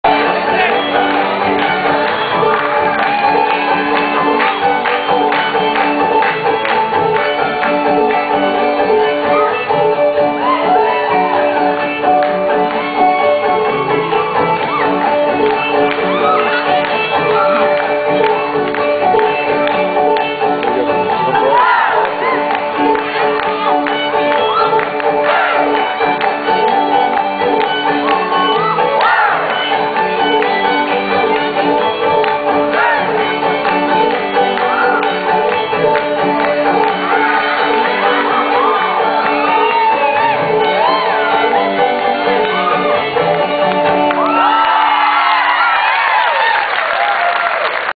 Horn In The West Dance
Opening night 2008. YeeeeHaw! This is what happens when actors get hold of Corn Squeezins...